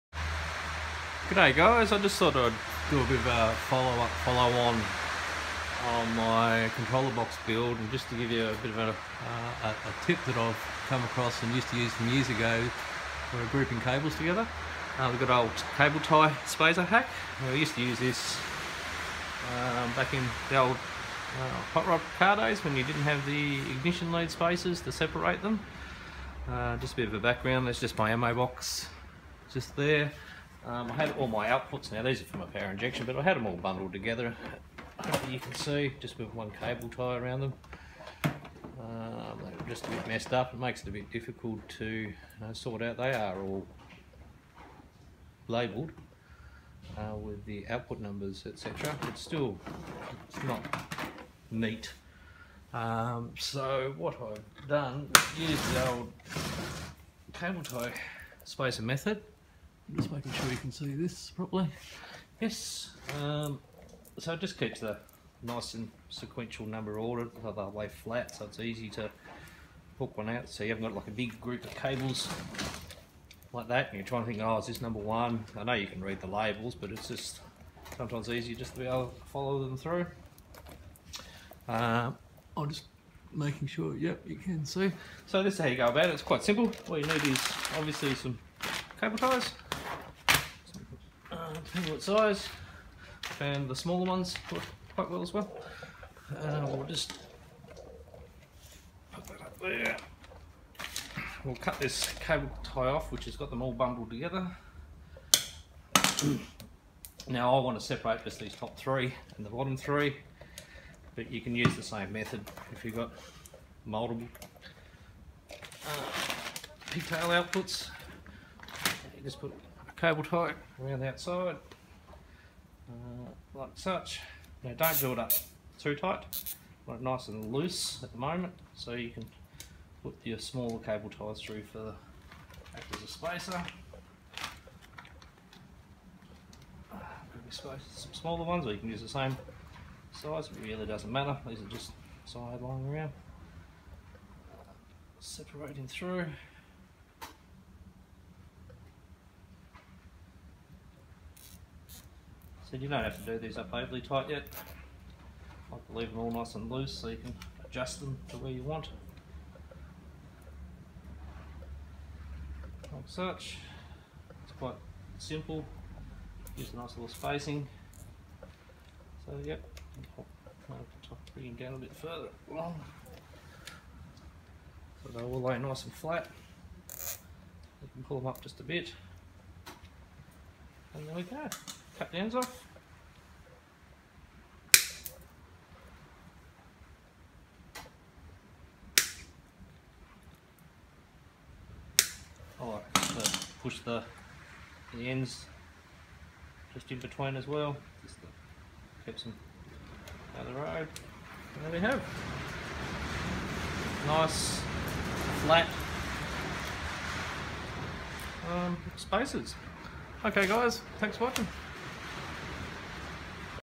Falcon F16 & F48 Pixel Controller Enclosure Build Part 7 - Cable Tie Spacer (Zip Tie) Tip
Part 6 - Just a quick tip I use for keeping the pigtails (outputs from pixel controller) neat and tidy using cable ties as a cable spacer.

Sorry for the poor audio it was raining really hard on the shed roof when I filmed this.

As a sneak peak at the similar controller enclosure I am going to use for my Falcon F16 V3 to control our Mega Tree.

Ammo Crate (Enclosure); Note for some reason these have had a price rise since I purchased them